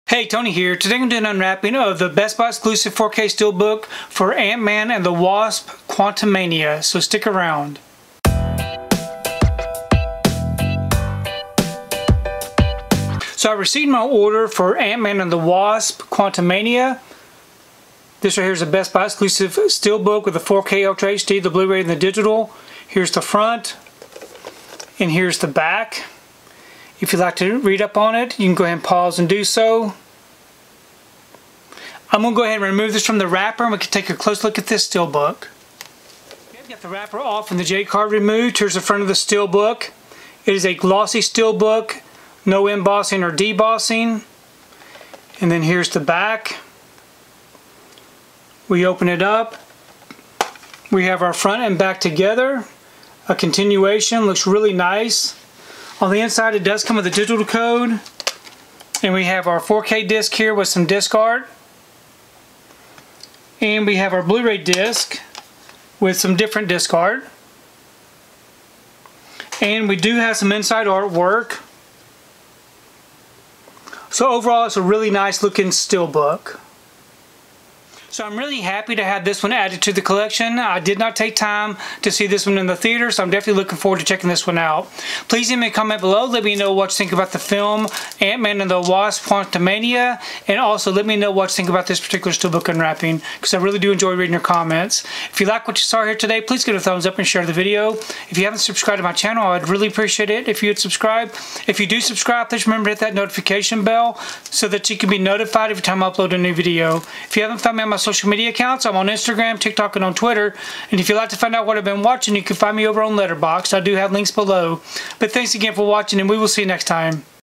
Ant-Man and the Wasp: Quantumania Best Buy Exclusive 4K Ultra HD Blu-ray Steelbook
Check out my unwrapping of the Ant-Man and the Wasp: Quantumania Best Buy Exclusive 4K Ultra HD Blu-ray Steelbook!

► Purchase the 4K Ultra HD + Blu-ray
► Purchase the Blu-ray